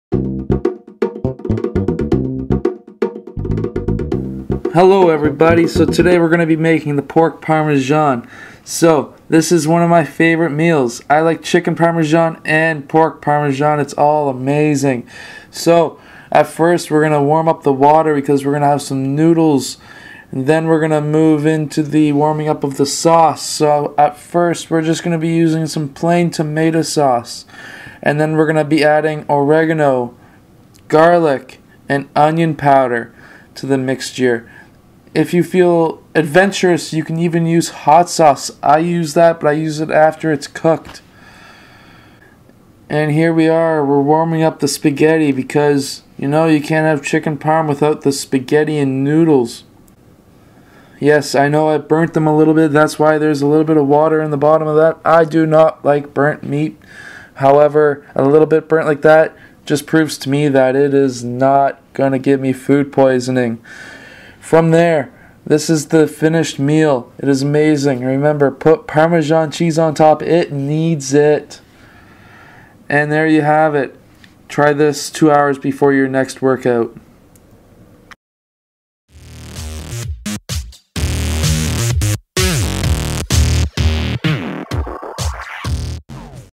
Pork and Chicken Parmesan Recipe Pre Workout Meal For Basketball Players
Pork and Chicken Parmesan is great as a pre workout meal, in this video you will learn how to cook the pork and chicken parmesan and a recipes for tomato sauce. This recipe I use as a pre workout before I workout or play Basketball.